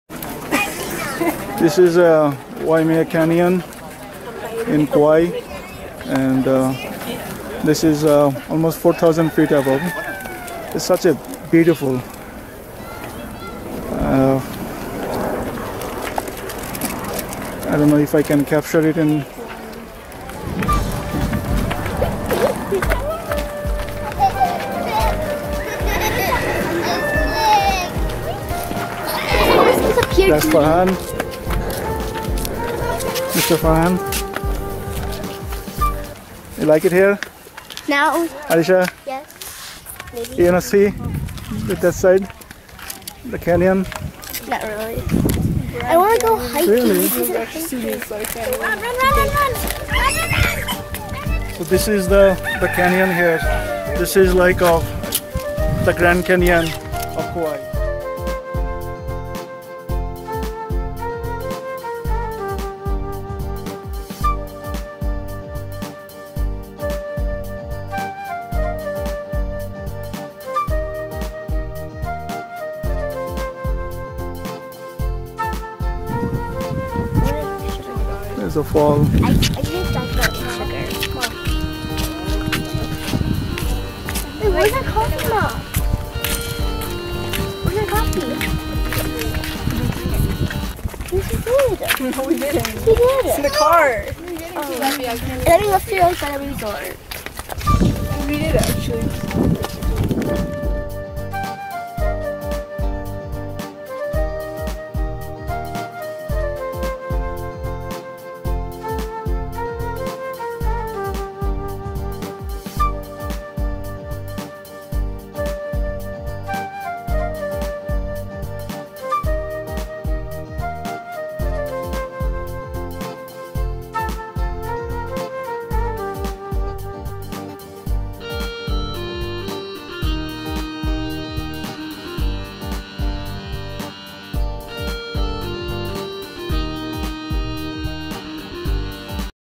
Discover Waimea Canyon in Kauai: Hawaii's Grand Canyon
🌄 Exploring Waimea Canyon - The Grand Canyon of the Pacific! 🌴

Hey there, travel enthusiasts! Join us on an unforgettable journey as we explore the breathtaking beauty of Waimea Canyon, also known as "The Grand Canyon of the Pacific," in Kauai, Hawaii. 🌺🏞️

We kicked off our adventure by renting a car and hitting the road in picturesque Kauai. The scenic drive to Waimea Canyon Lookout was nothing short of spectacular! 🚙💨

As we ascended to over 4,000 feet, the panorama that unfolded before our eyes was absolutely jaw-dropping. The vivid colors of the canyon painted an otherworldly landscape that left us in awe. 🌄😲

But the real gem was the hidden waterfall near Waimea Canyon Lookout. The sight of the cascading waters was nothing short of magical, and we couldn't resist capturing the beauty on camera! 📸🏞️

Join us as we take you on this remarkable journey through Waimea Canyon, a natural wonder also referred to as "The Grand Canyon of the Pacific." 🌟📽️